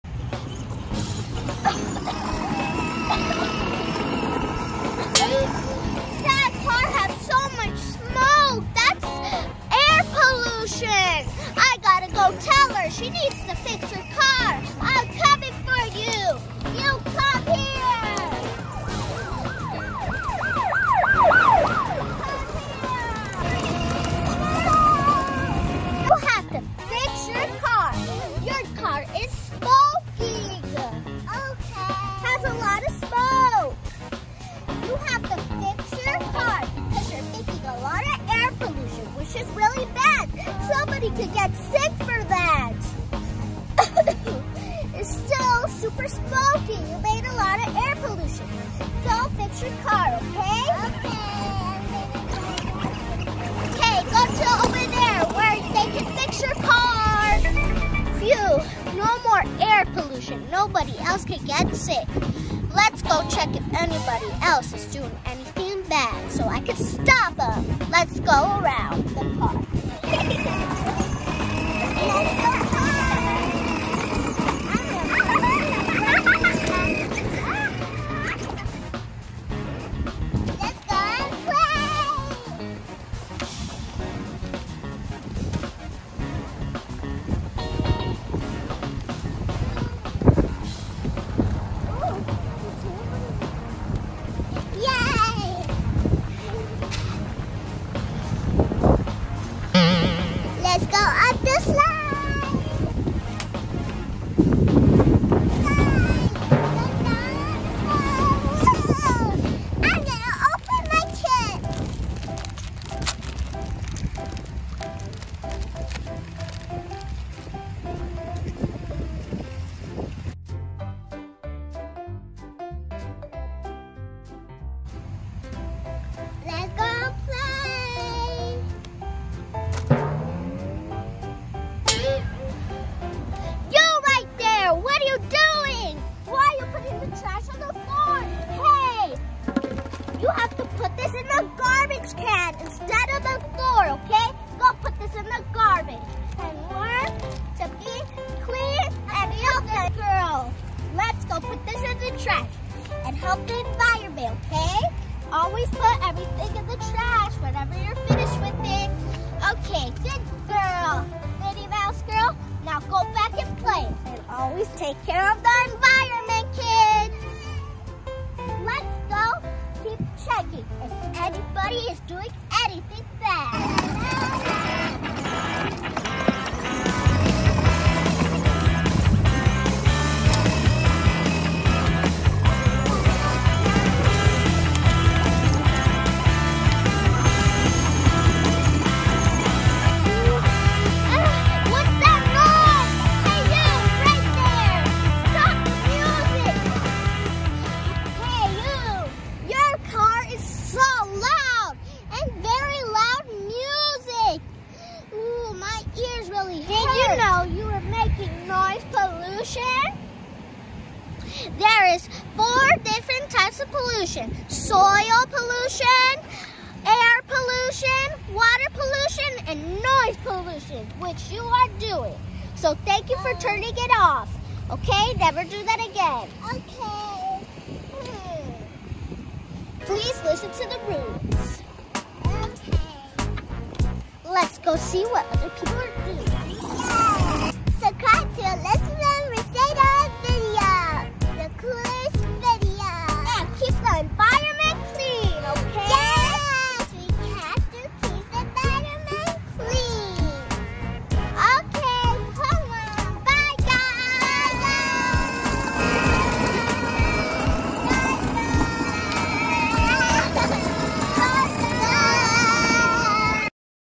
How to Take care of the Environment | Good Habits for Kids | Play Safety | Educational Videos.
Kids videos for kids play safety how to take care the environment good habits for kids.